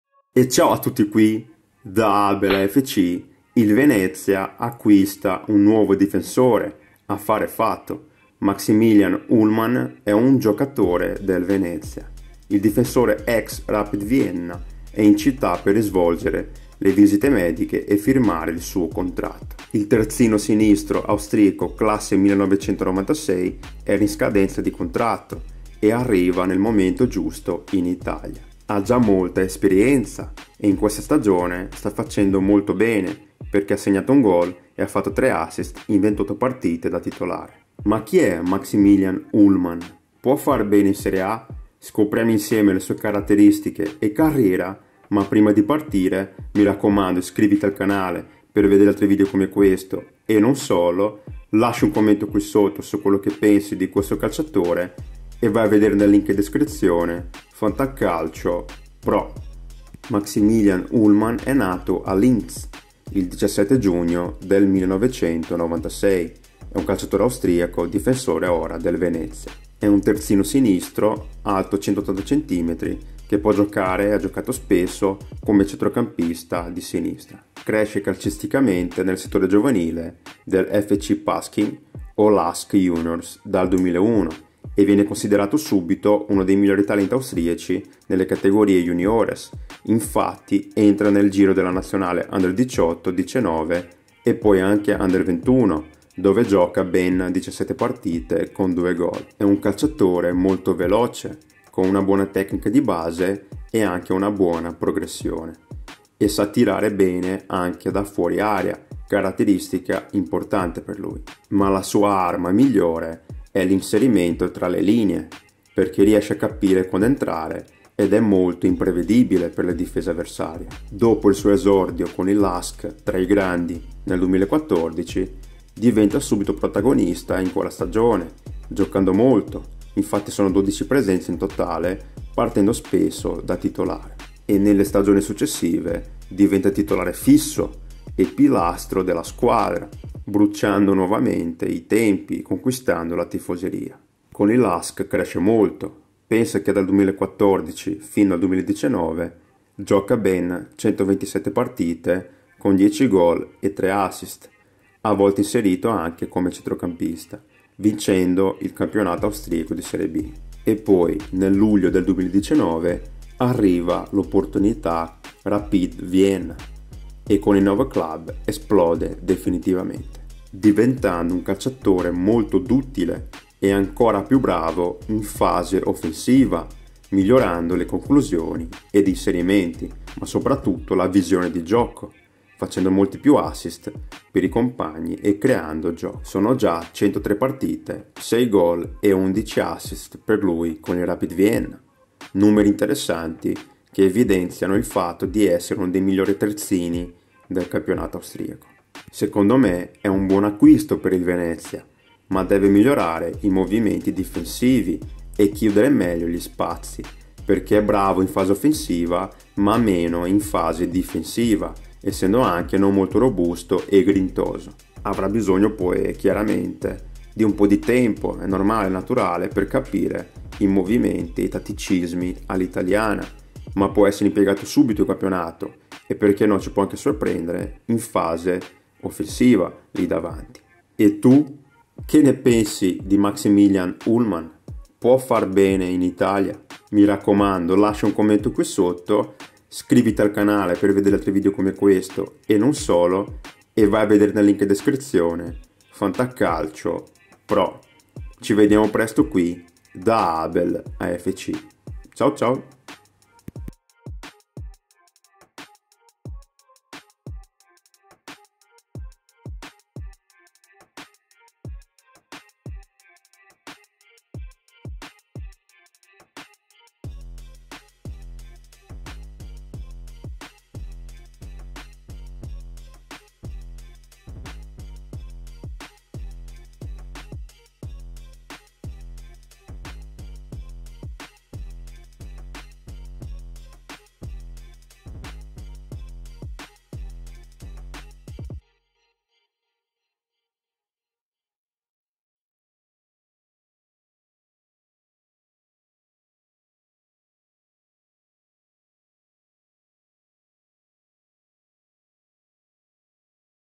Chi è MAXIMILIAN ULLMANN? VENEZIA CHIUDE PER IL TERZINO DEL RAPID VIENNA!
Maximilian ULLMANN è il nuovo difensore del Venezia. Terzo acquisto di gennaio. Scopriamo insieme le sue caratteristiche e carriera ⚽😱😝